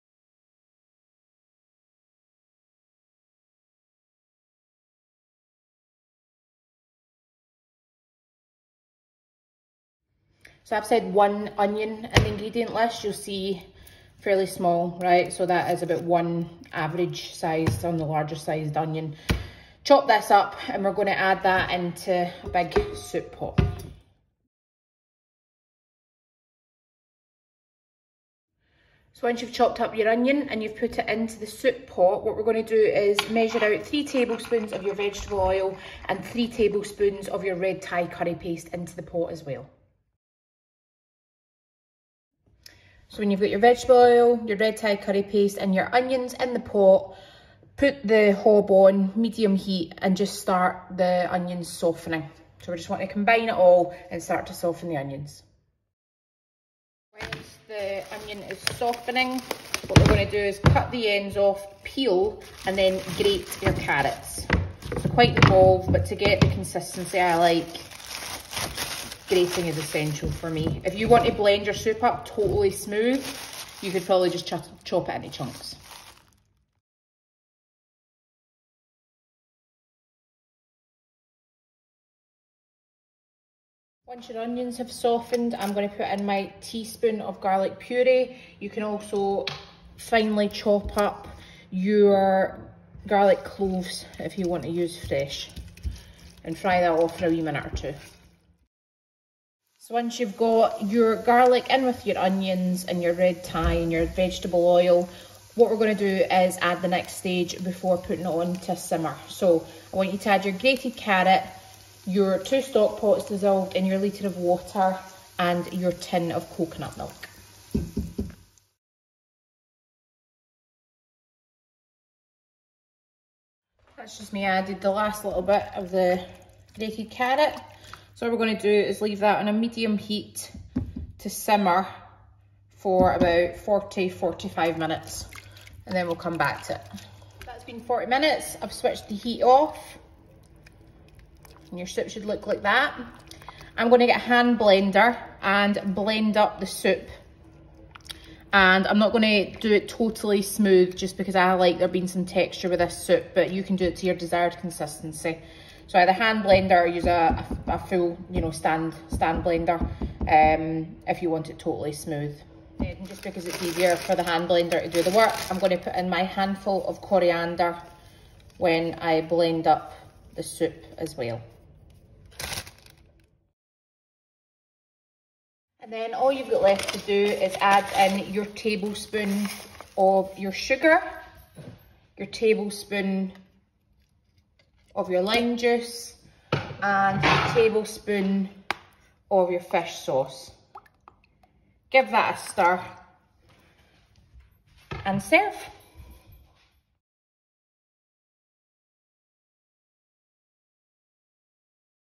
RED THAI CARROT & COCONUT SOUP RECIPE
- 3 x tbsp vegetable oil
- 1 x onion
- 3 x tbsp red Thai curry paste
- 2 x cloves garlic or 1 x tsp purée
- 1kg x carrots (peeled and grated
- 2 x veg or chicken stock pot/cube dissolved in 1L water
- 1 x tin coconut milk
- 1 x tsp salt
- 1 x tbsp fresh lime juice
- 1 x tbsp sugar
- 1 x tbsp fish sauce
- handful coriander

Method
1. Finely chop onion and add to soup pot with vegetable oil and red Thai curry paste- cook until softened
2. Whilst onions are softening, cut ends off, peel and grate carrots
3. Add garlic (finely chopped, crushed or grated), cook for another 1-2mins
4. Add grated carrot, coconut milk and stock pot/cube dissolved in water, cook for 40mins on medium heat
5. Use stick hand blender to blend to desired consistency
6. Add fish sauce, lime juice and sugar to soup and taste if it requires any seasoning (salt/pepper)